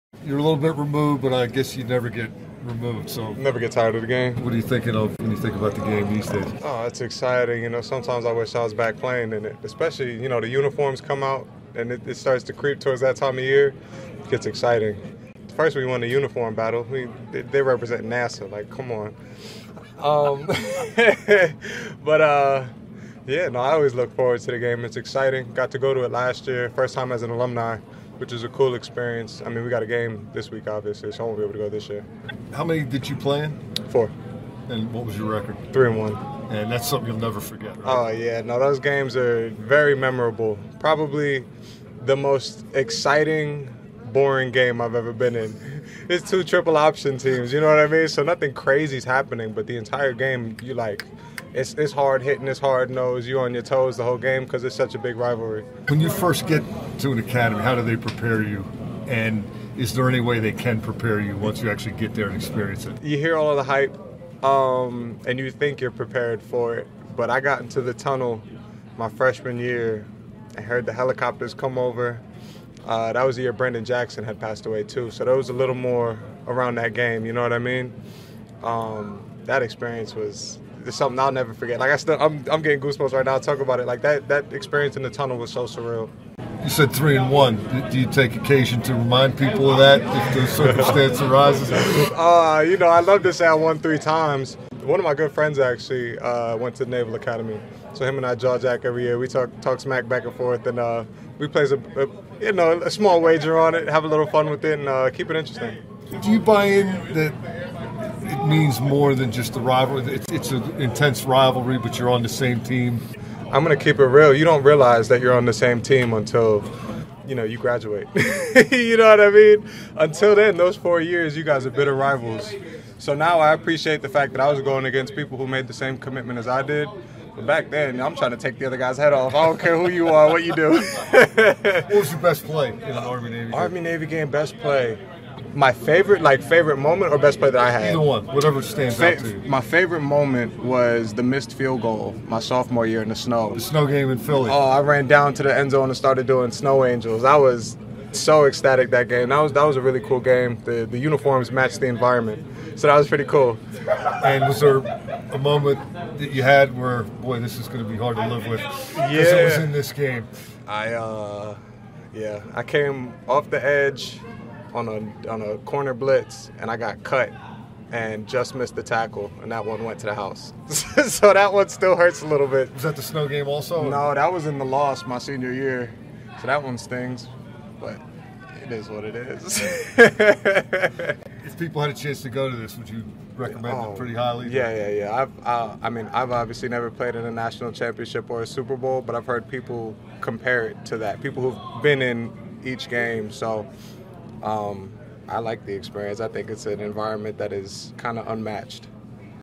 Mike Pristua - Elijah Riley - ‘An environment that is kinda unmatched’
As a graduate of the United States Military Academy, Steelers practice squad safety Elijah Riley appreciates Army-Navy more than most. The rivalry that resumes Saturday in Philadelphia is unlike any other.

 📸: Getty Images